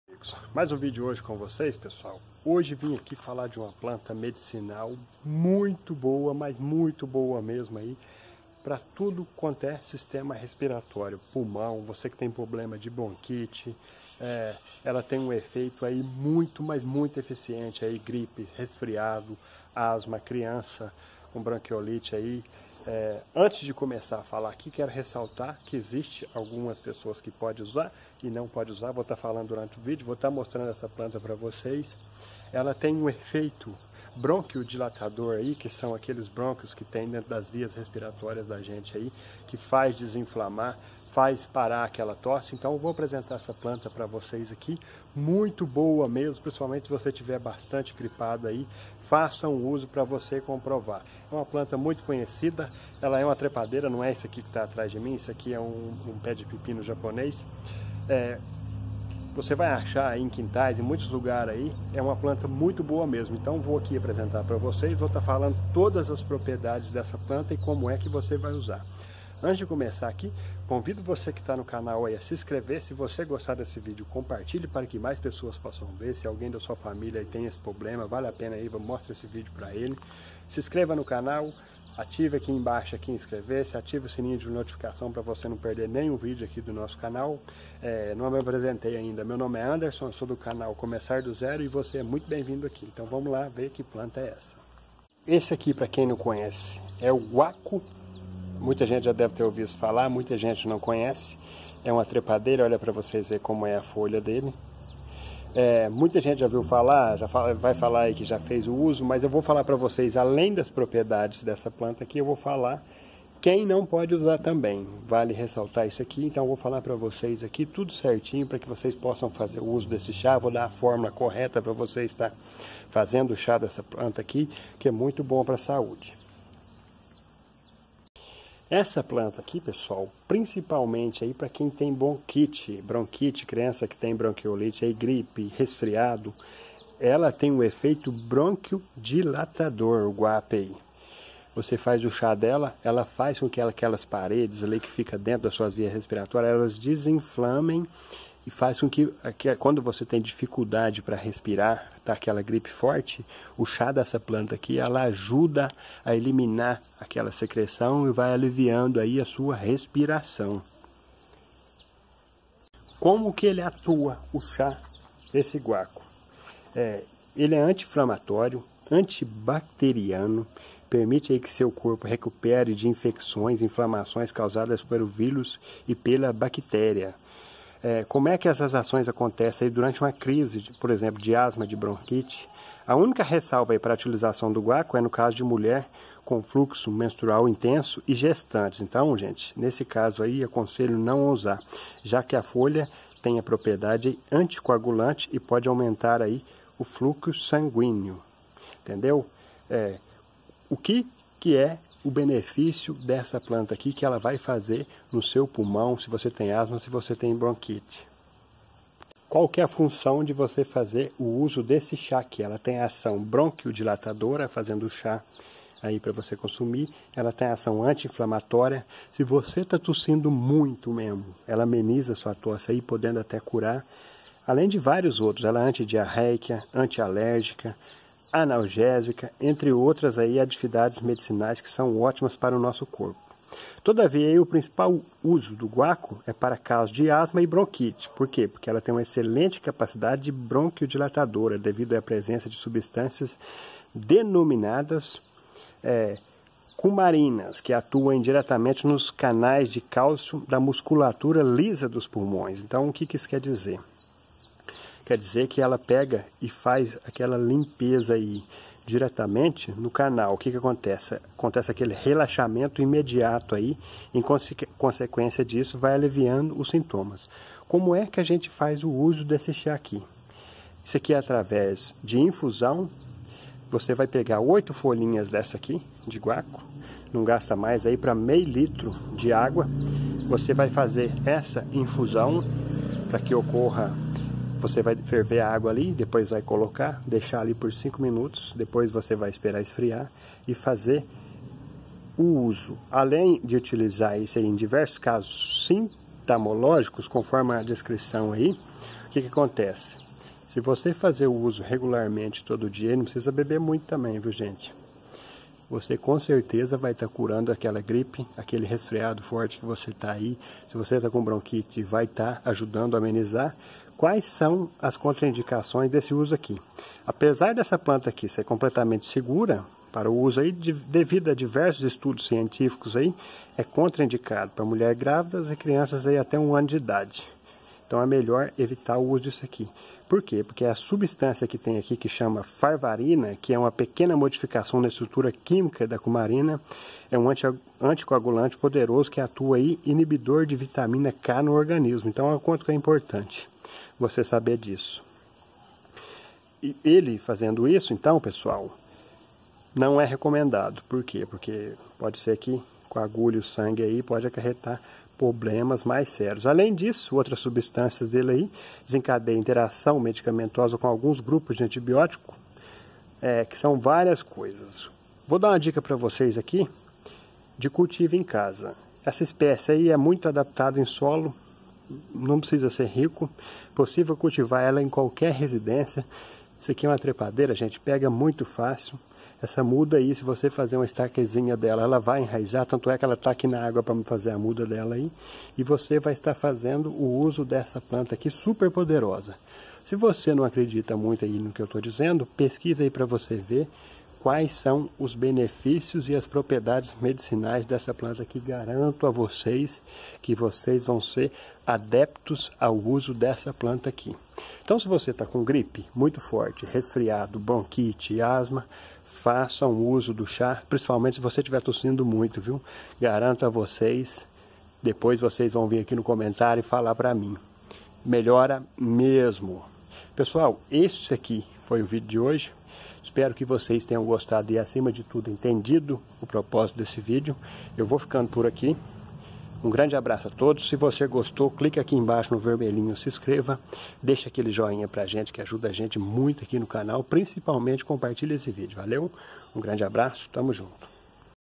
GUACO:MELHORA BRONQUITE,ASMA,TOSSE E AUMENTA SISTEMA IMUNOLÓGICO
Olá conheça o GUACO ESSA PLANTA MELHORA VIAS RESPIRATÓRIAS COMO POR EXEMPLO PULMÕES CARREGADOS , BRONQUITE,ASMA,TOSSE CRÔNICA,E ALEM DE TUDO ISSO AUMENTA SEU SISTEMA IMUNOLÓGICO VALE A PENA CONFERIR

Participe do nosso grupo do telegram

Conheça nossa página no Facebook venha fazer parte dessa grande família